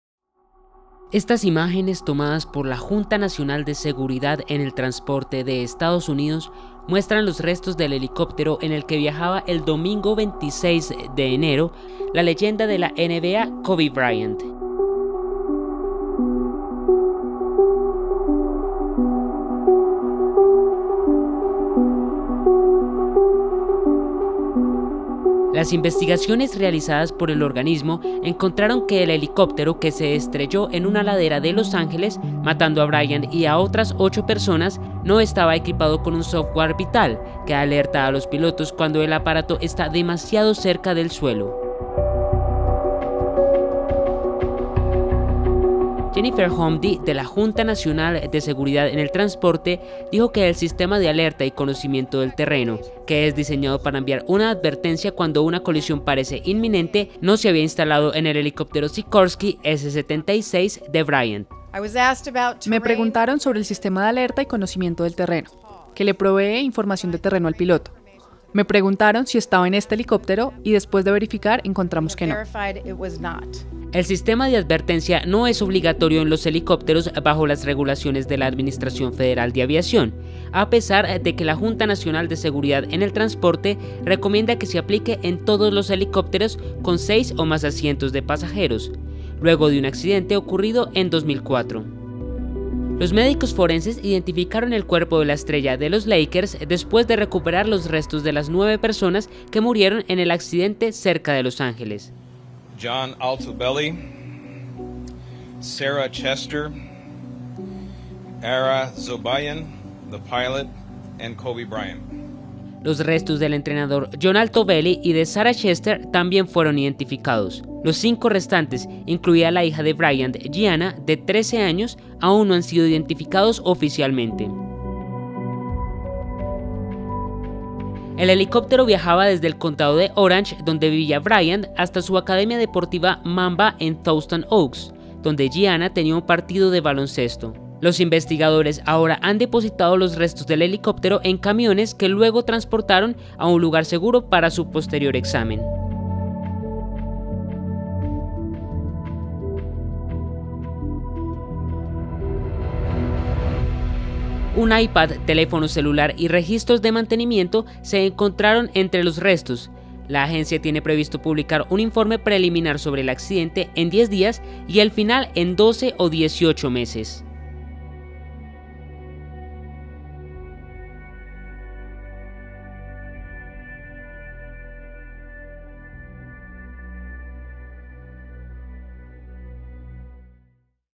Kobe Bryant: su aeronave no tenía sistema de alerta de riesgo de colisión - El Espectador
La Junta Nacional de Seguridad en el Transporte de EE.UU. reveló las imágenes de los restos de helicóptero en el que falleció la estrella de la NBA, Kobe Bryant. Los investigadores hallaron que la aeronave no tenía el sistema de alerta y conocimiento del terreno que advierte a los pilotos cuando hay riesgo de colisión.